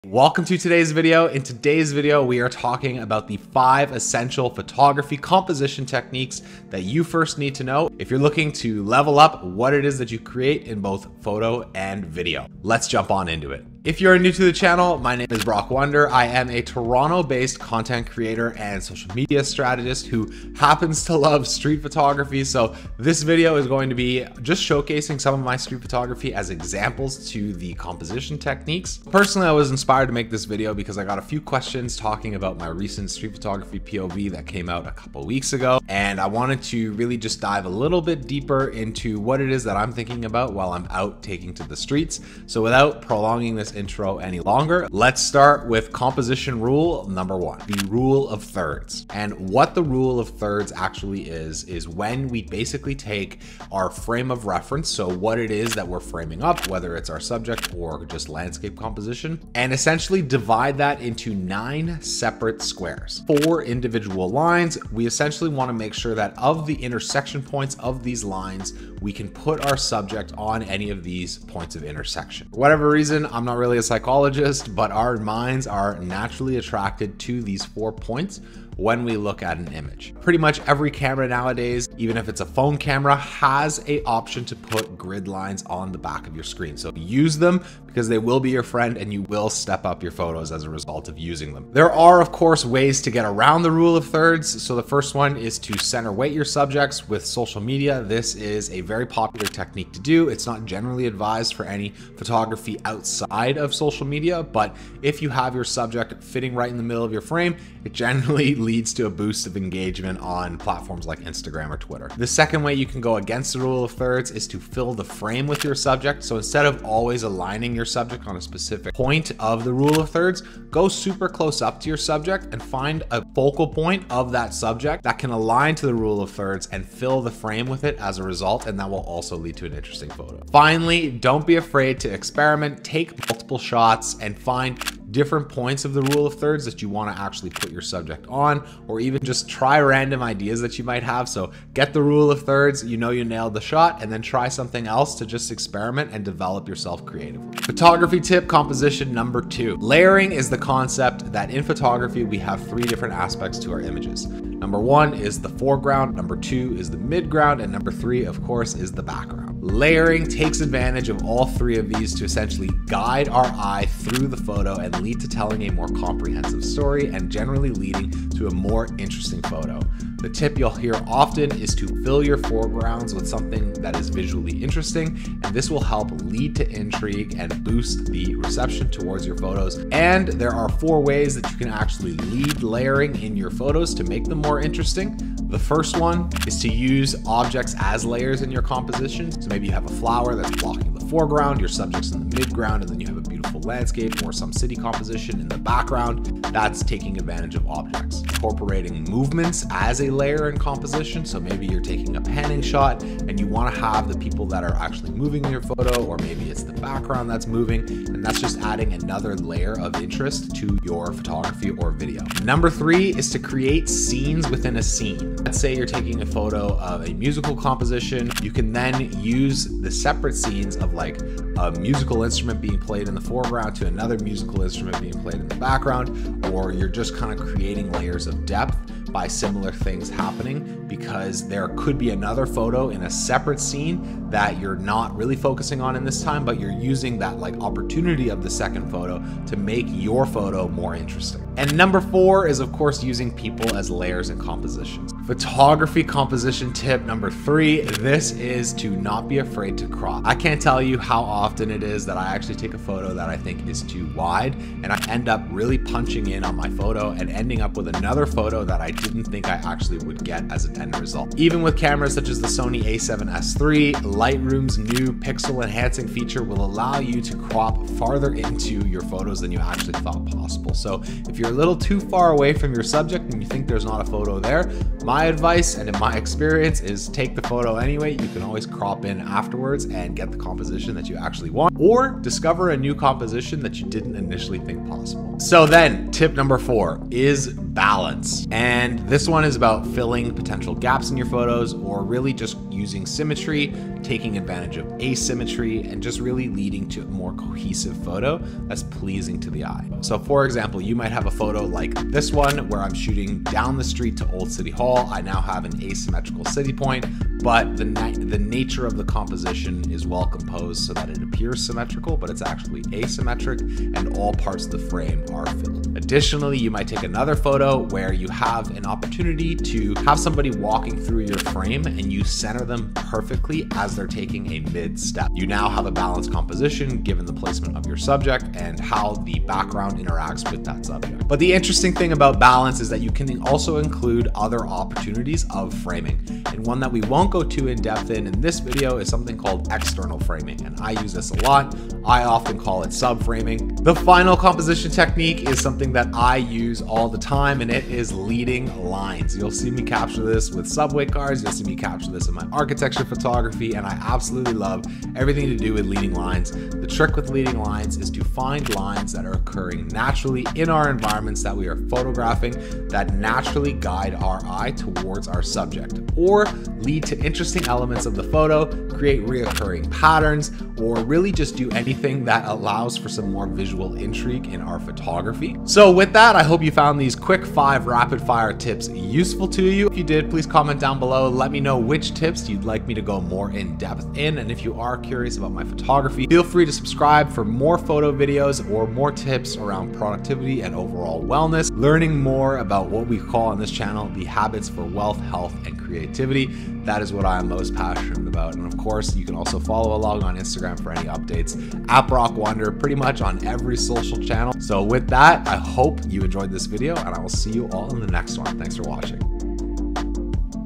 How To Take Better Photos - Composition Tips & Tricks!
We break down the essential compositions that both photographers and videographers need to master in order to confidently capture eye-catching photos and videos!

Composition is one of the most essential components of any good creator so these tips will go a long way in helping you level up what it is you can capture!

Let me know if you would like a dedicated video for any of these in the comments below.

@brockwunder on all platforms